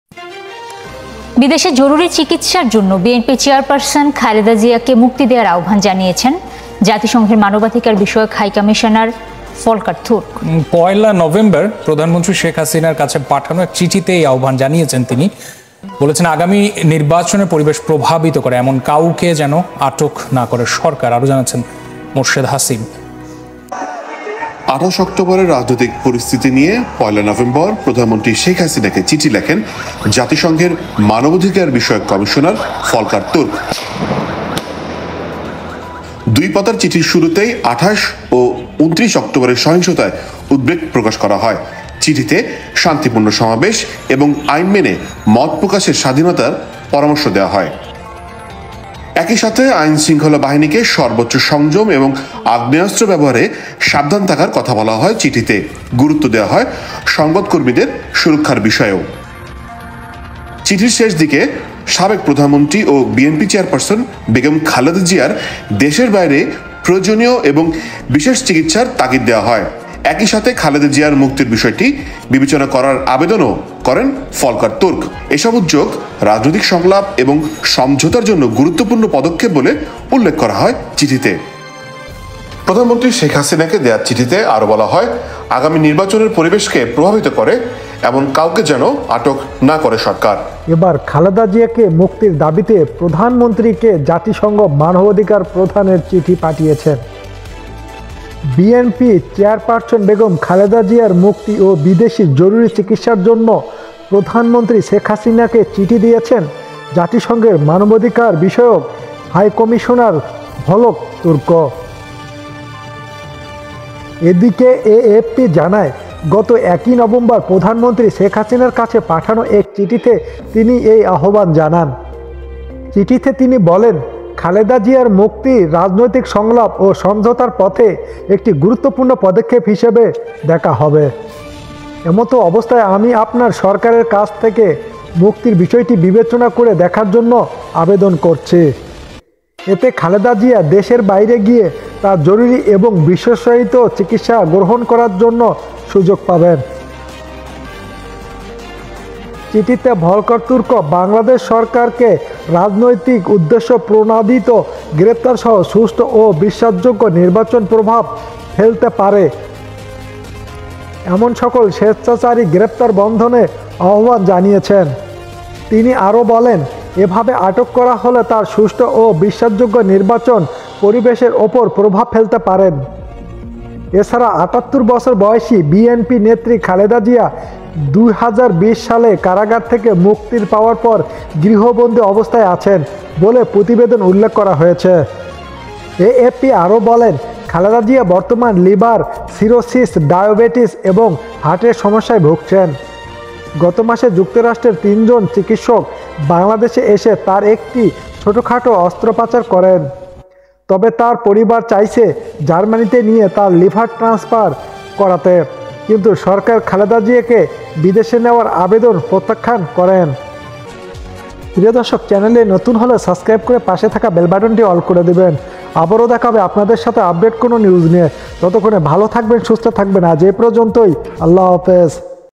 এইমাত্র খালেদা জিয়ার কারাগার থেকে মুক্তির সংবাদ। খালেদা জিয়ার মুক্তি দাবিতে জাতিসংঘের চিঠি।
এইমাত্র খালেদা জিয়ার কারাগার থেকে মুক্তির সংবাদ। খালেদা জিয়ার মুক্তি দাবিতে জাতিসংঘের চিঠি।bdnews